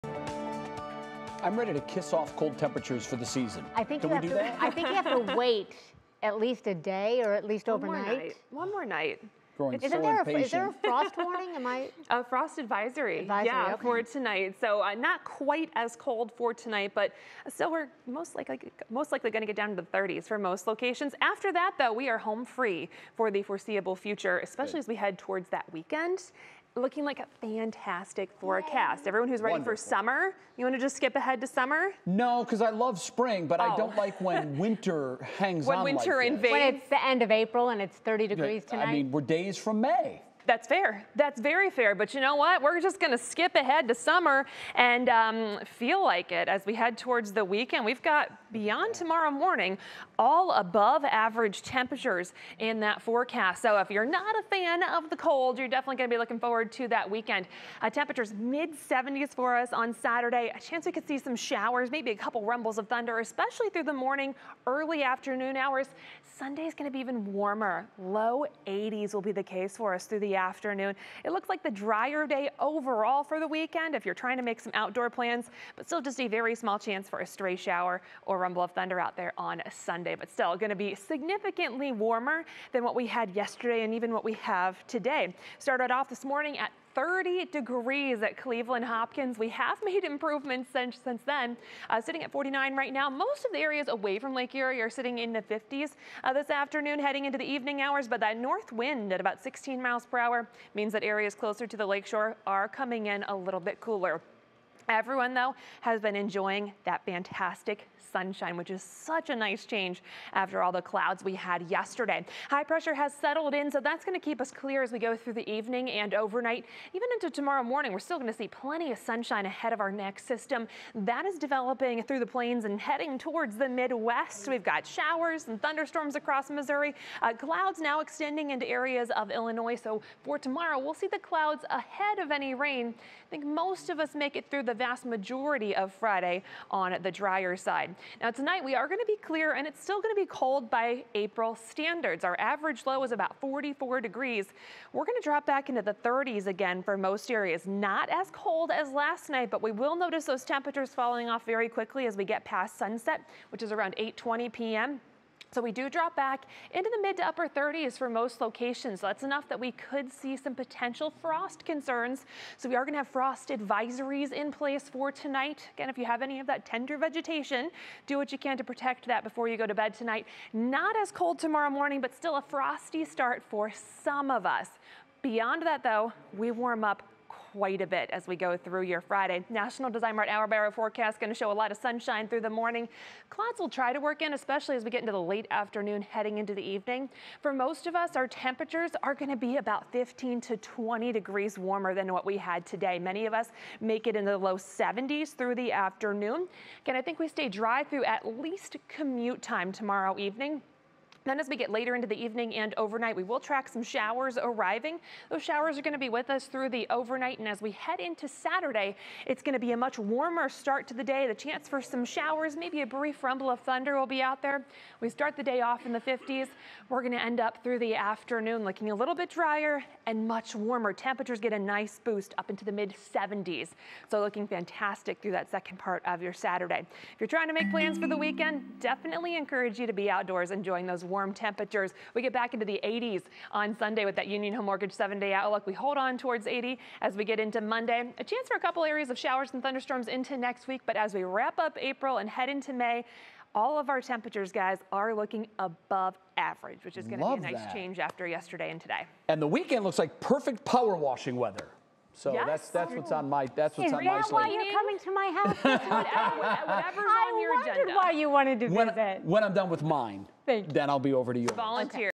Northeast Ohio weather: Summer-like temperatures on the way
Your 5 p.m. Greater Cleveland and Northeast Ohio weather forecast for April 6, 2024:

We've taken a step in the right direction today and temperatures will continue to improve. Mainly clear tonight with patchy frost with temperatures in the mid to upper 30s. Sunshine for much of Friday with clouds arriving later on. Highs near 70. Mainly dry through the daylight hours. Rain arrives later in the evening and into the overnight.

A few showers/rumbles possible mainly Saturday morning. Highs in the mid 70s. Summer warmth for Sunday with highs near 80 with just a small chance for a stray shower/storm.

Staying above average into next week for the end of April and beginning of May. A few chances for showers/storms, but temperatures should be able to stay decent.